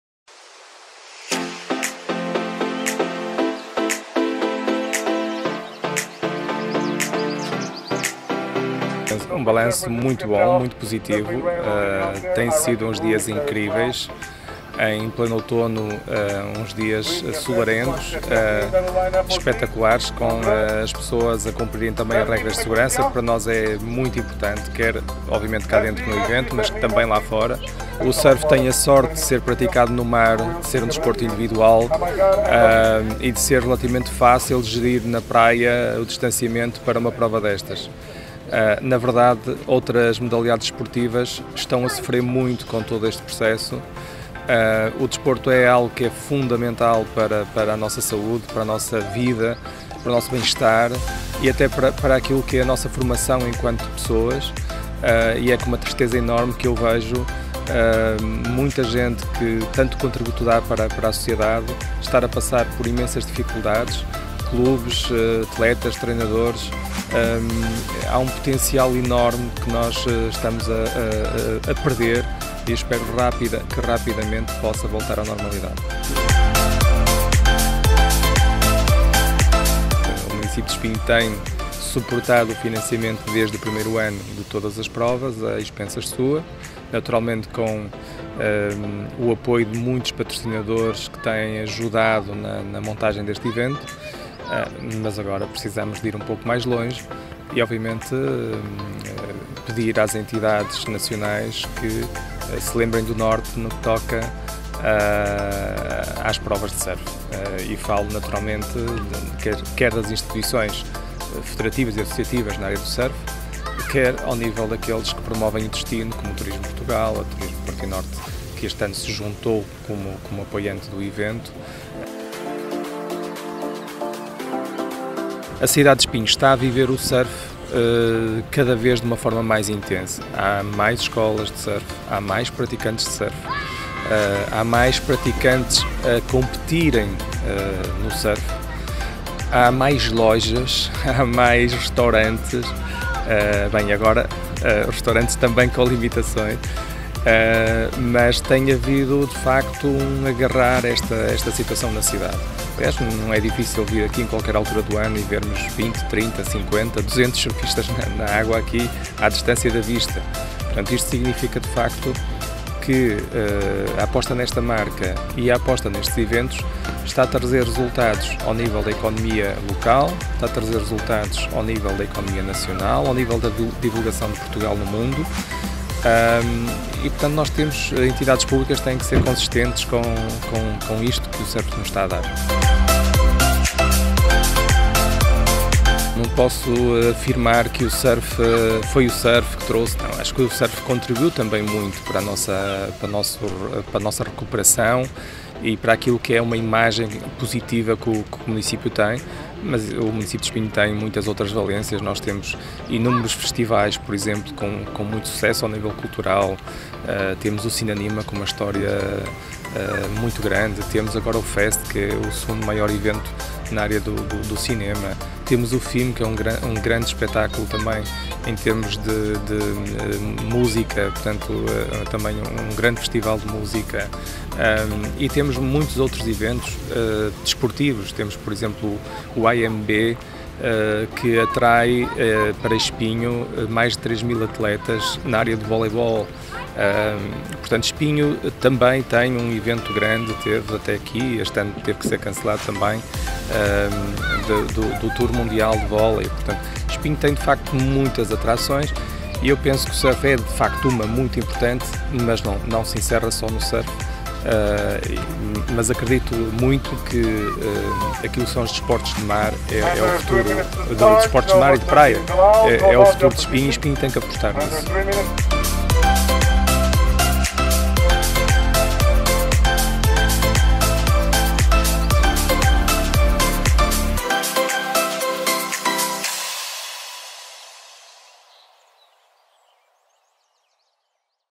"Espinho Surf Destination" com balanço positivo
Entrevista com Vicente Pinto, Vice Presidente da Câmara Municipal de Espinho, após o evento "Espinho Surf Destination, que decorreu de 6 a 11 de outubro na Praia da Baía.